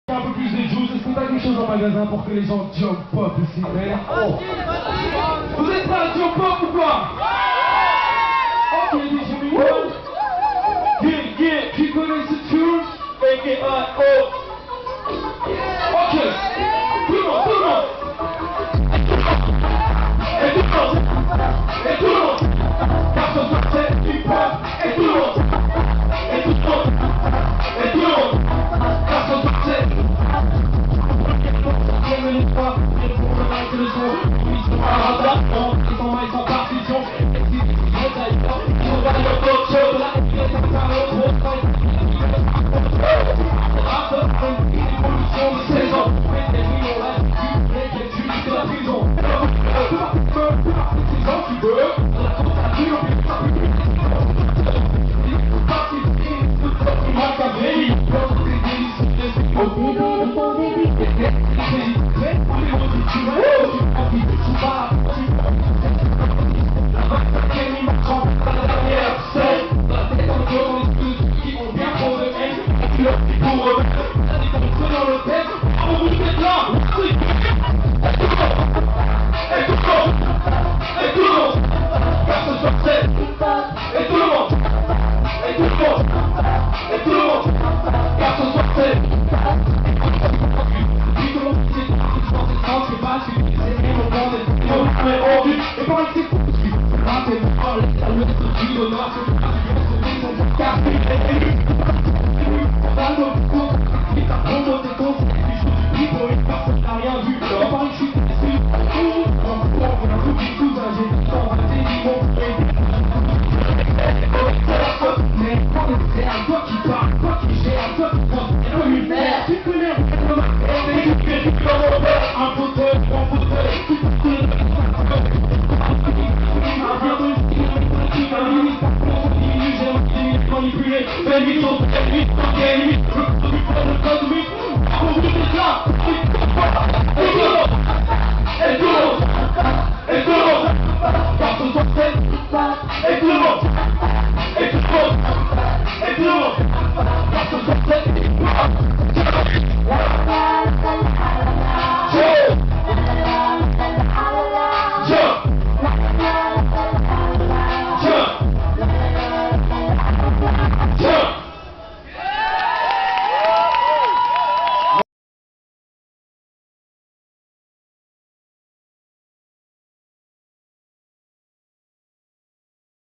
Akro-fête de la musique 2007
Un deuxième morceau d'AKRO à la fête de la musique 2007, Barvaux